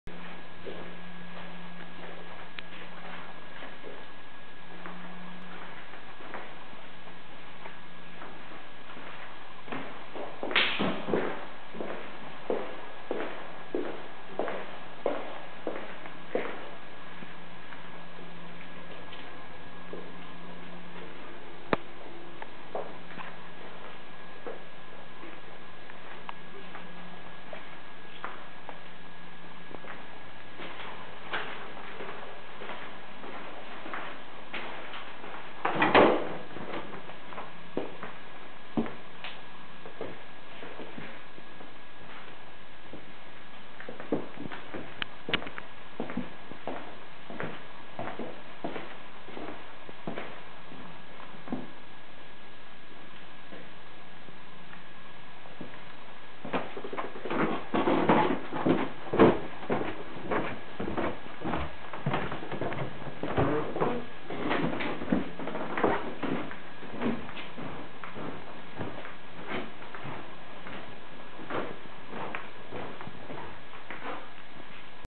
Vila de inchiriat langa Hotel Royal
All imobiliare Constanta inchiriaza Vila P+ 1, 6 camere, partial mobilata, 3 bai, 2 bucatarii, 2 centrale termice, proaspat renovata, teren 200 mp, posibilitate parcare auto in curte, 799 E/ luna, o luna in avans si una garantie